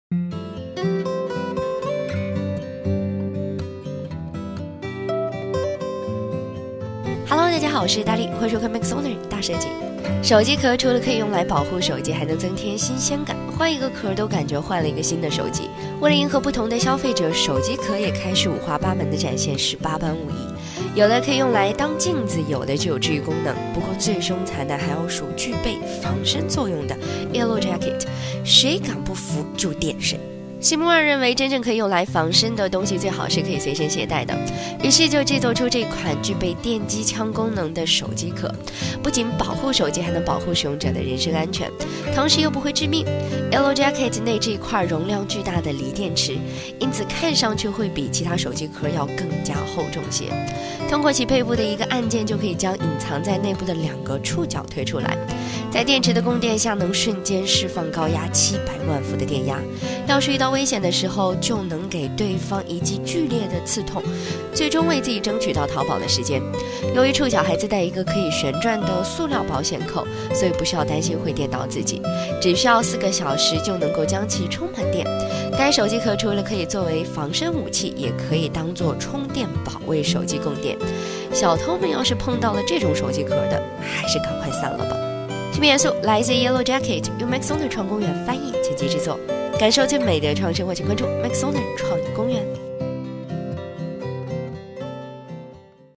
全球最凶残手机壳，瞬间放电700万伏，让坏人有来无回
手机壳除了可以用来保护手机，还能增添新鲜感，换一个壳都感觉像换了一个手机，为了迎合不同的消费者，手机壳也开始五花八门的展现十八般武艺，有的可以用来当镜子，有的具有治愈功能，不过最凶残的还要数具备防身作用的Yellow Jacket，谁敢不服就电谁。